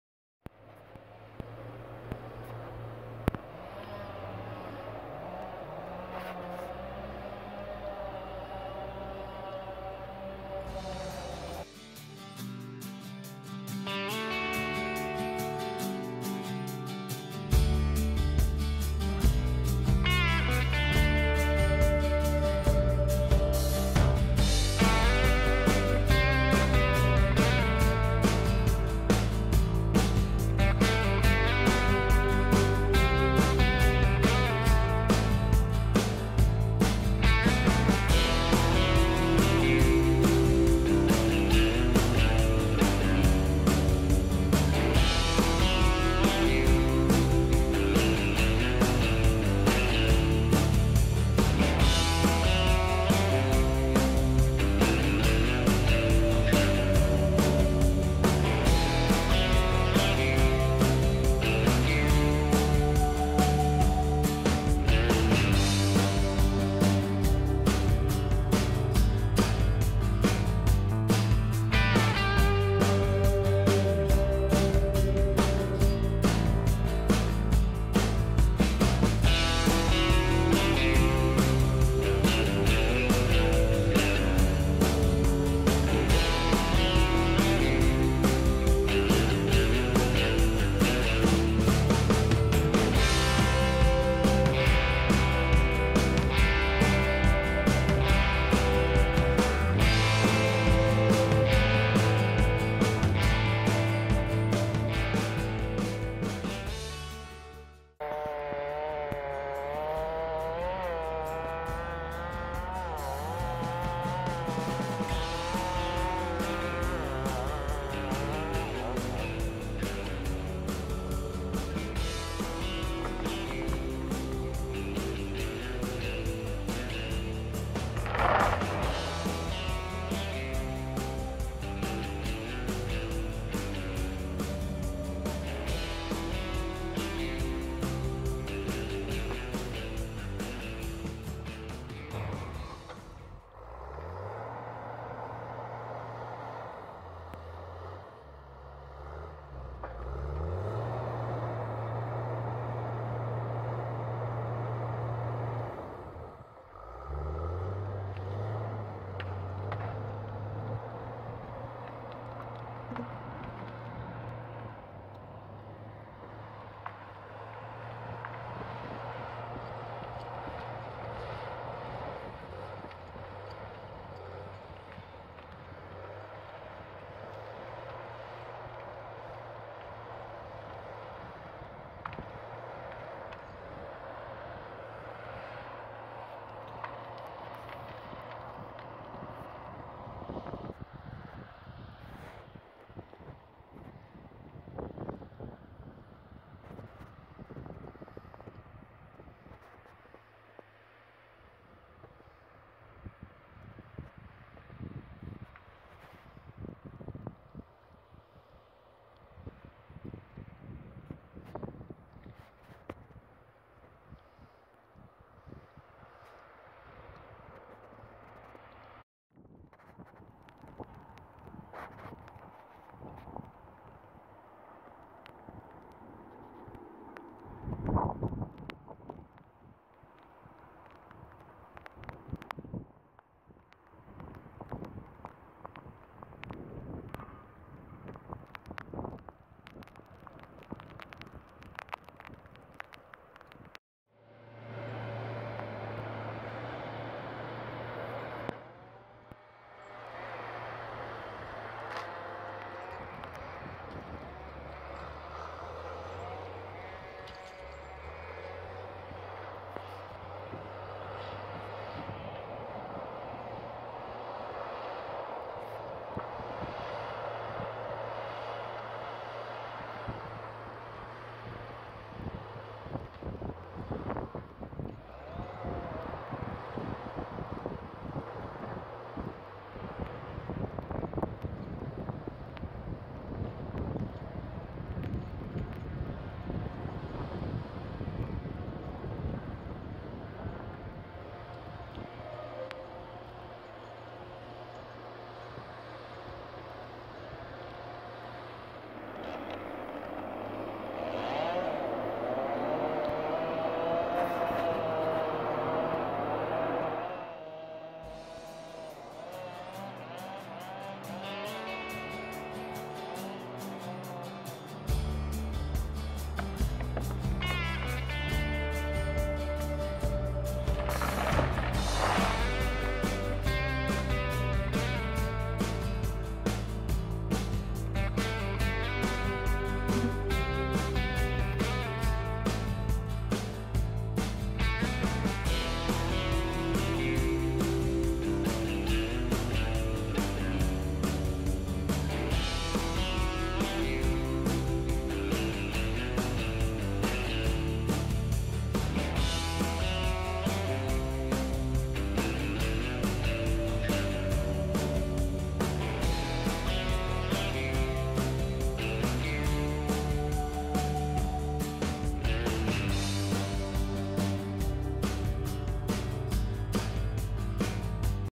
Hello sunshine just kidding
Justa a little more logging and tree falling in a wet spring was a nice day fly the drone next day snow